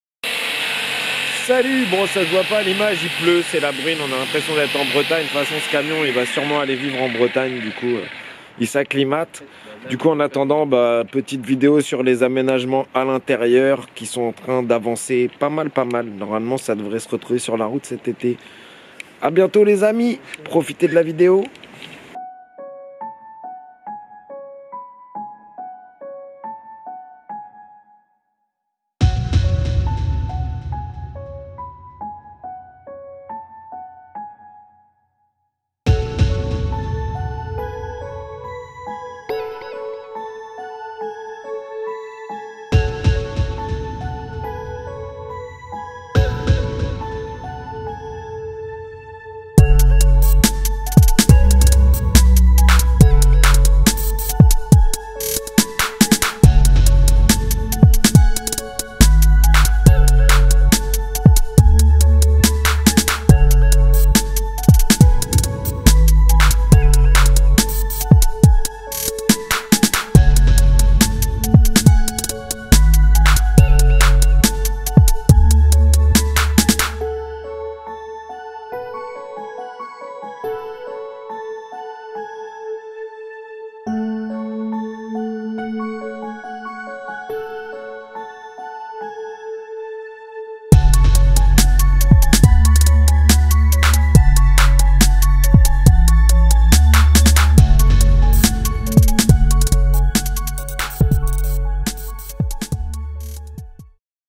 le mercedes 508D de lolo episode 10
quelques images de l'aménagement intérieur
en particulier le plan de travail et l'évier
bientôt prêt pour la grande aventure